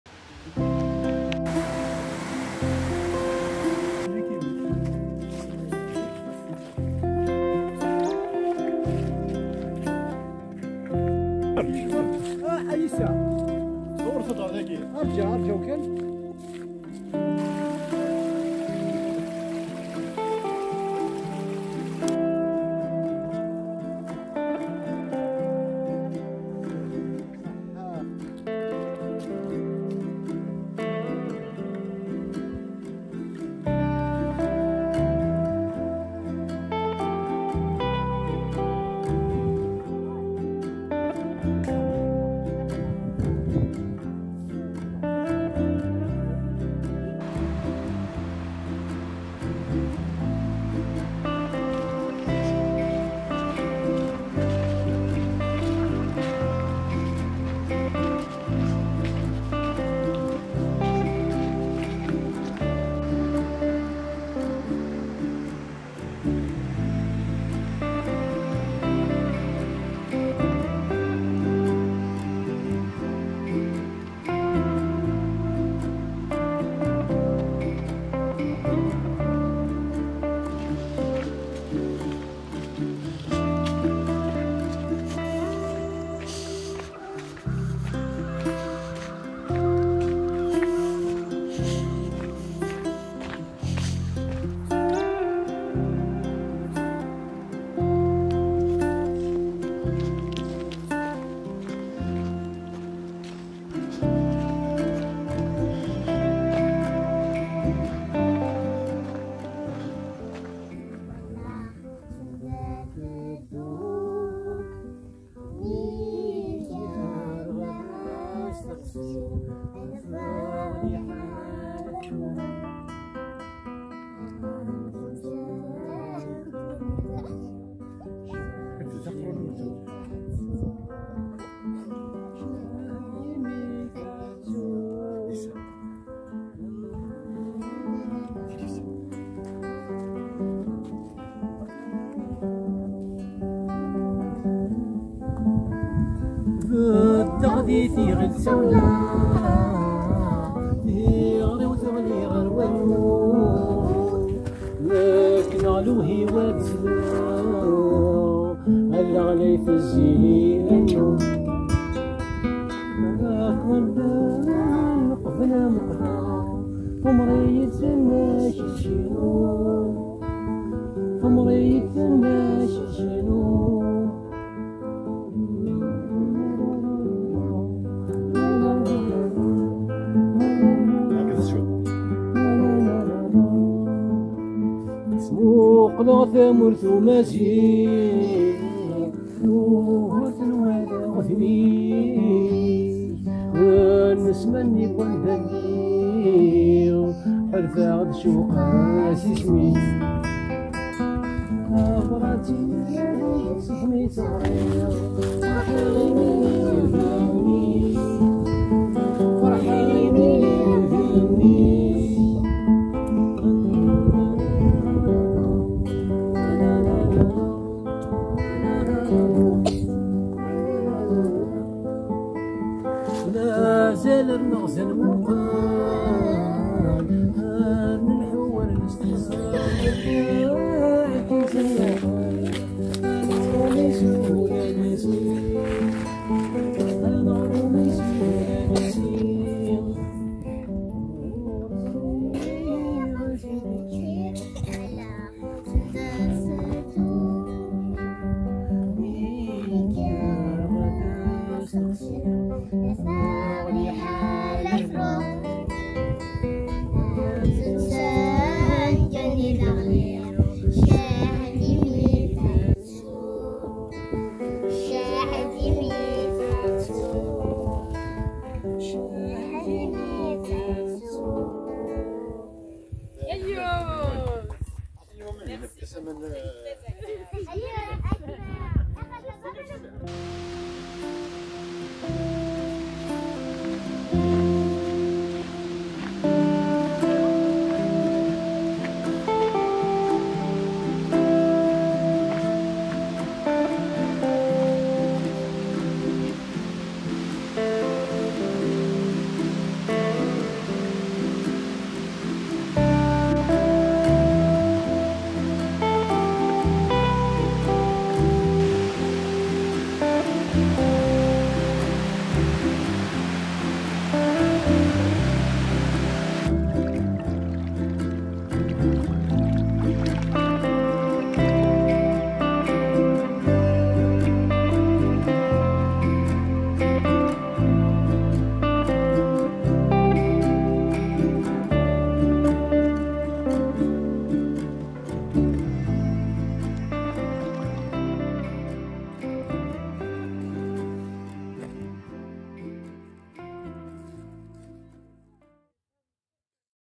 Randonnée pédestre
Randonnée pédestre kabyle.
Circuit Lac Meech -Chalet Western / au cœur du Parc de la Gatineau .